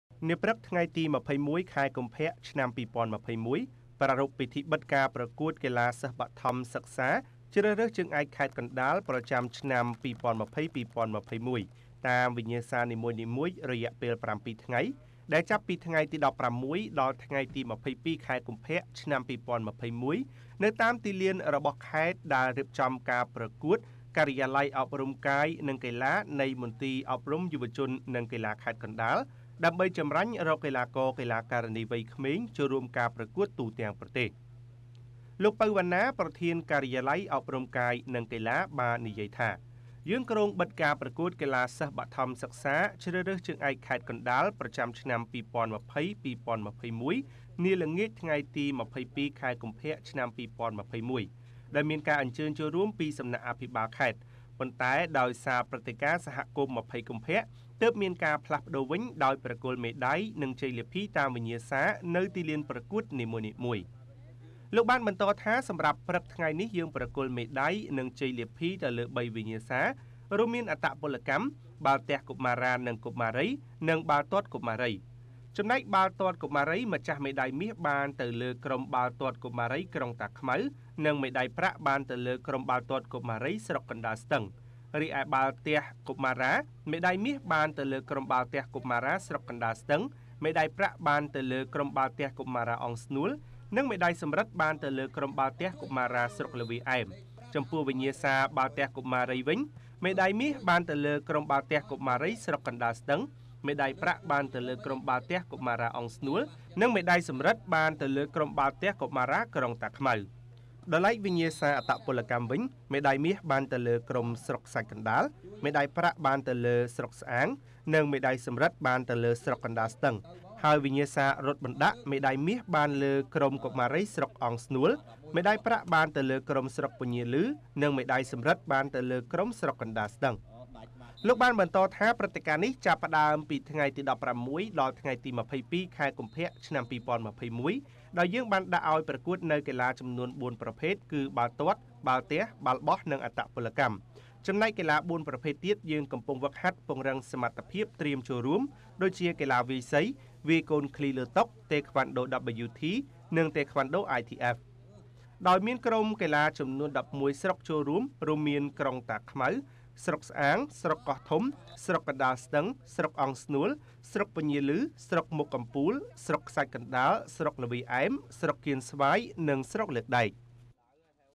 ខេត្តកណ្តាលចម្រាញ់អត្តពលិកវ័យក្មេងបីវិញ្ញាសារ ចូលរួមការប្រកួតសិស្សបឋមទូទាំងប្រទេសប្រចាំឆ្នាំ ២០២១
ខេត្តកណ្តាលចម្រាញ់អត្តពលិកវ័យក្មេងបីវិញ្ញាសារដើម្បីចូលរួមការប្រកួតសិស្សបឋមទូទាំងប្រទេសប្រចាំឆ្នាំ ២០២១